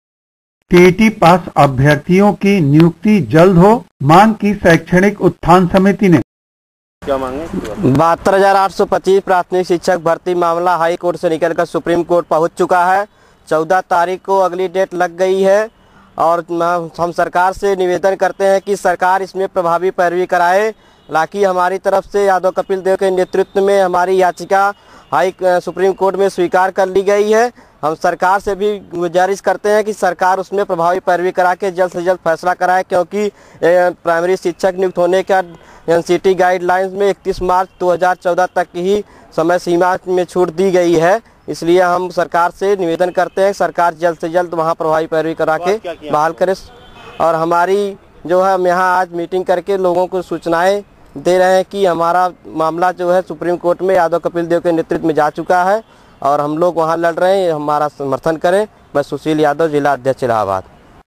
newsallahabad 12 2 14 2
मांगी टीईटी पास लोगों की नियुक्ति की मांग की शैक्षणिक मेरिट उत्थान समिति ने